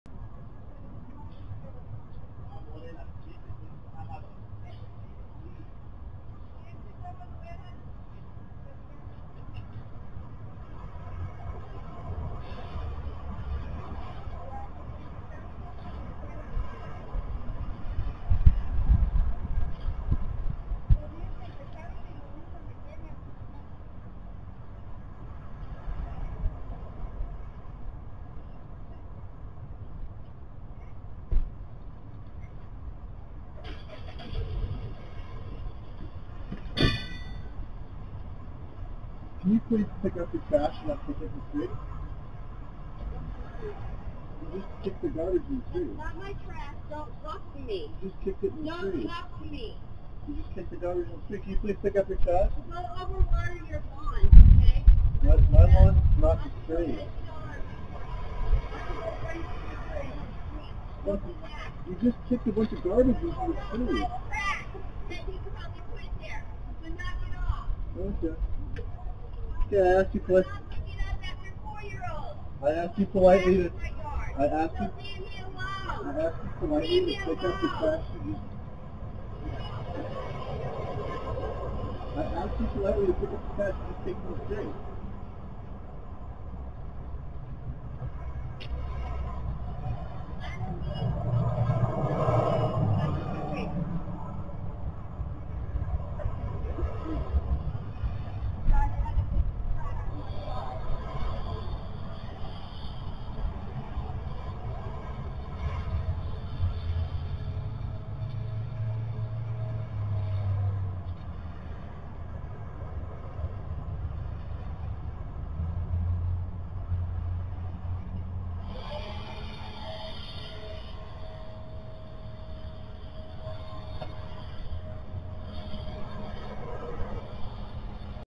Crazy Veronica litters and is asked to pick up trash
In this video you can see crazy veronica kicking her trash into the street. i come out and ask her to pick it up and she refuses and yells and then blows it more into the street with her leaf blower